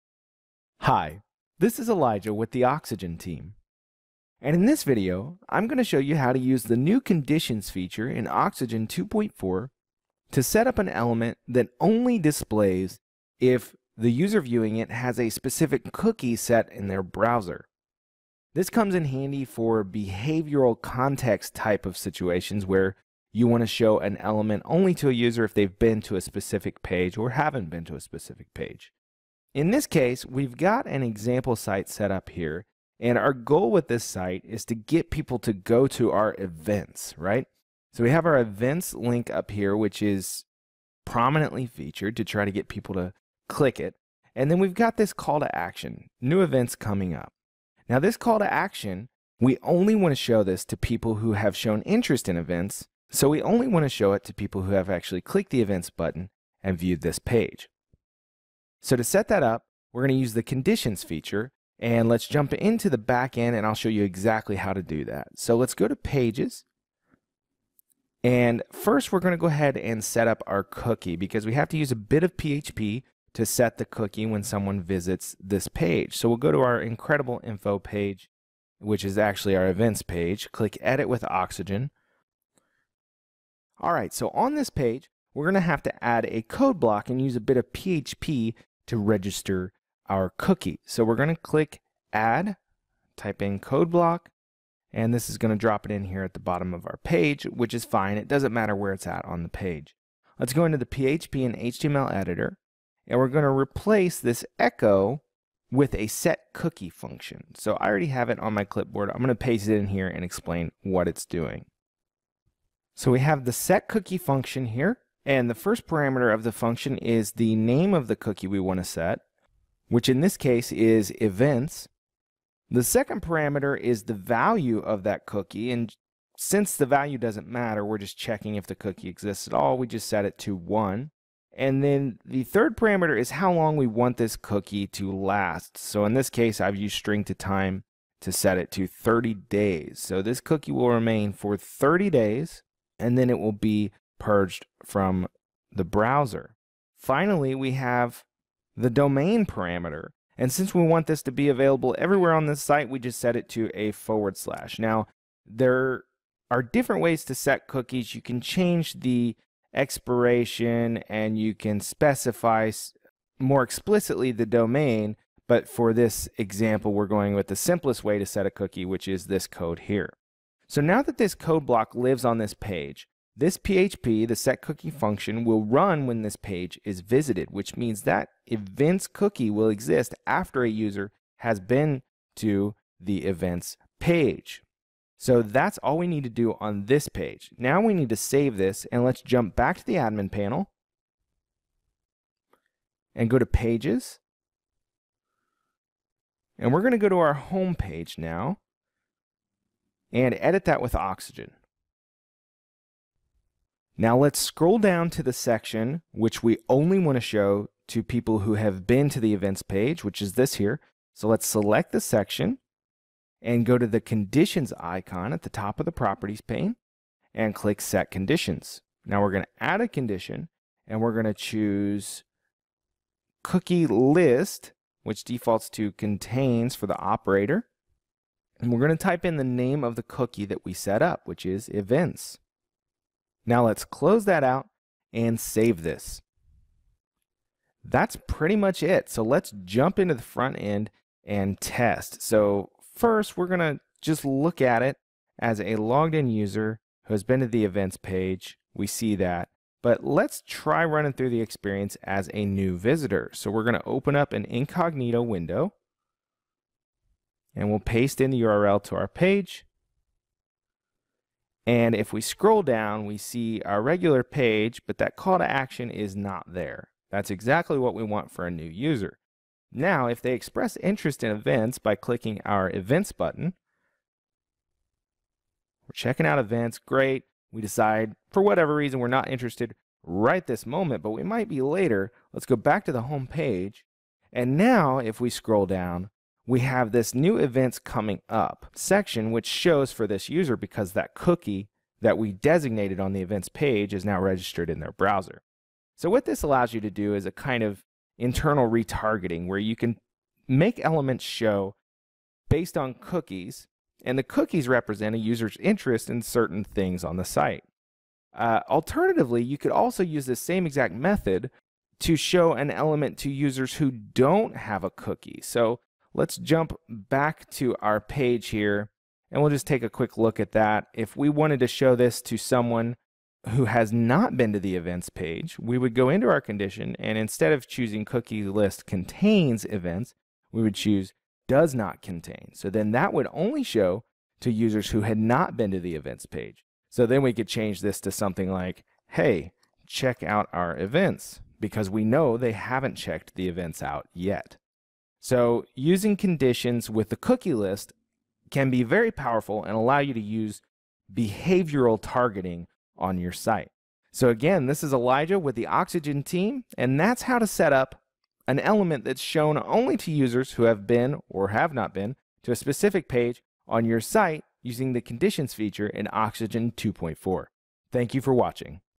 Oxygen Conditions Feature - Show Content Based on Cookies
In this video, learn how to use the Conditions feature in Oxygen to show/hide an element on the page using a cookie set from another page on the site.